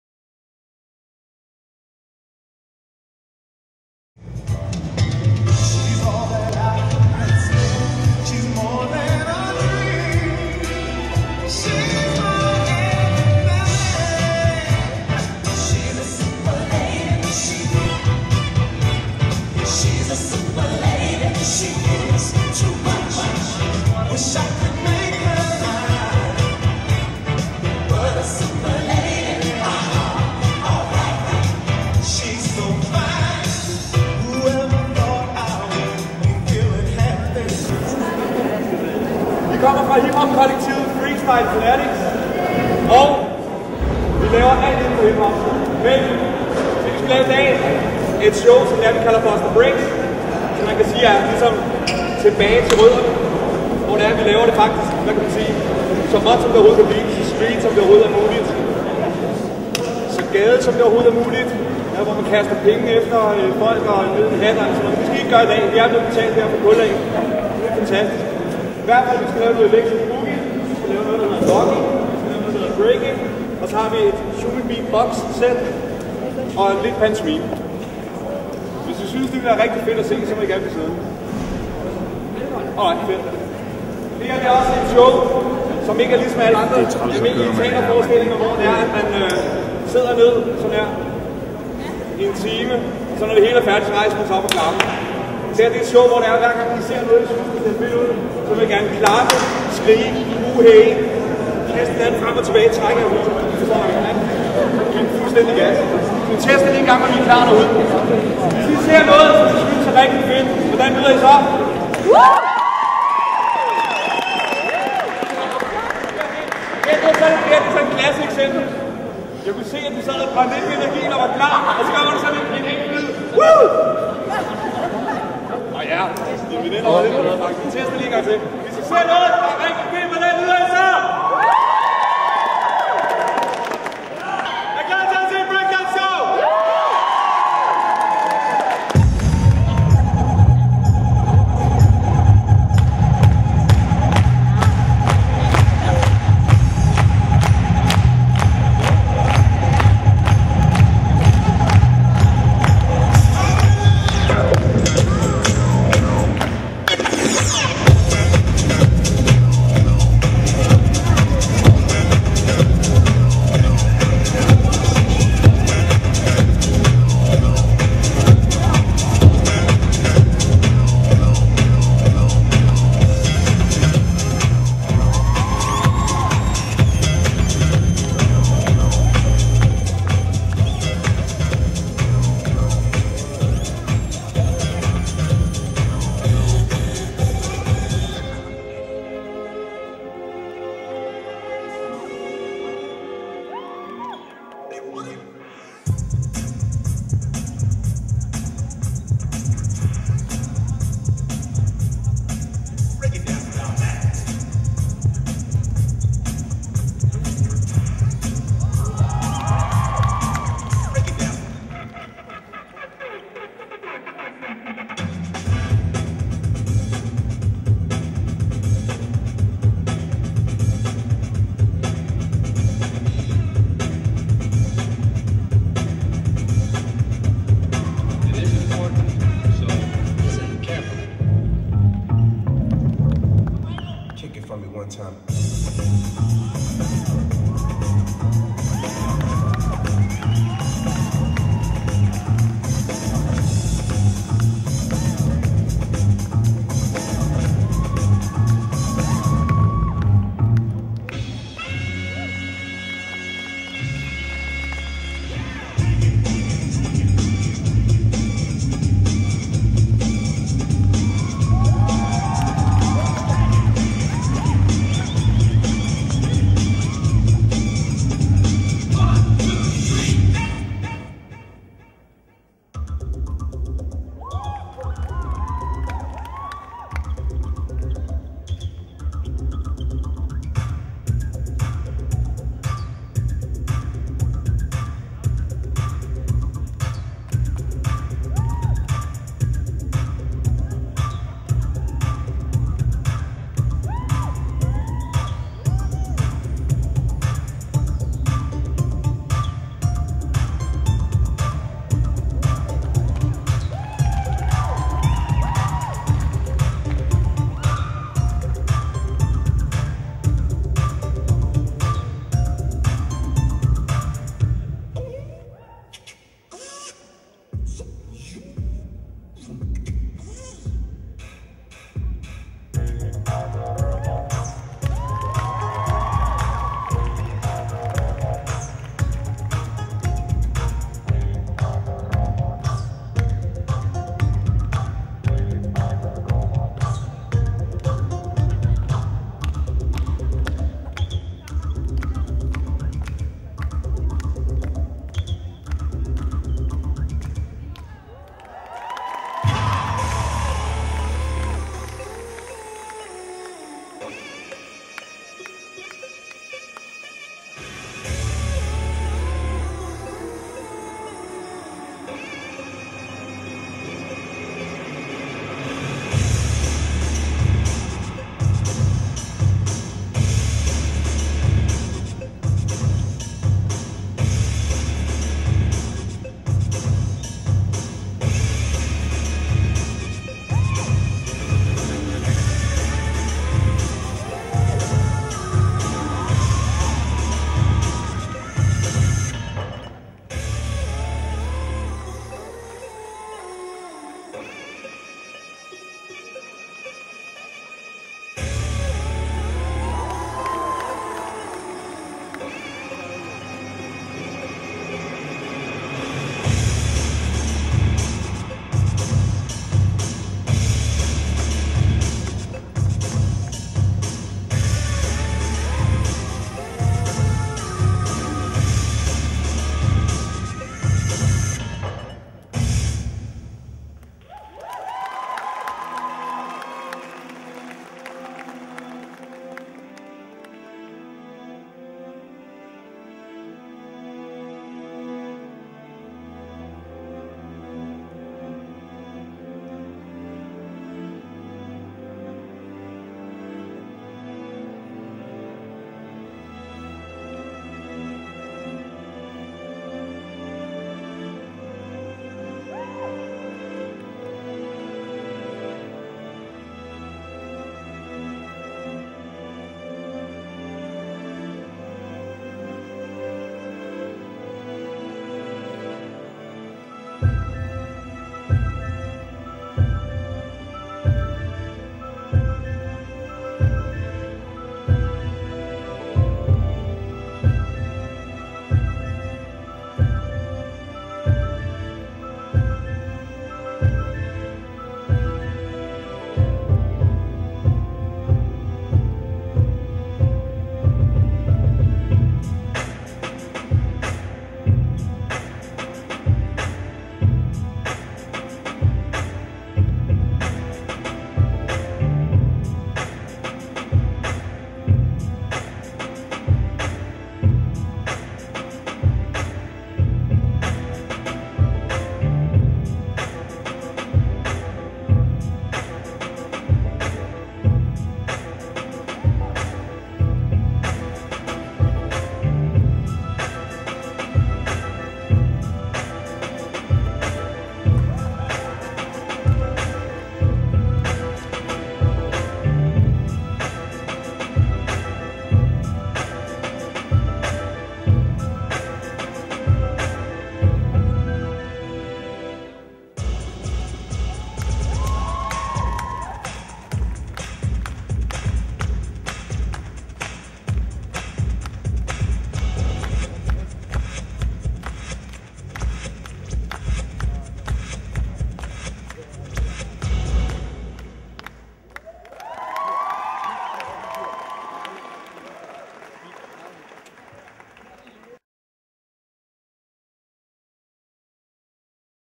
HipHop-Kollektivet Freestyle Phantastix - Del 1
HipHop Kollektivet Freestyle Phantastix optrådte i Nordkraft med et forrygende Break- og Streetdanceshow - Station 10 var selvfølgelig med - Første del!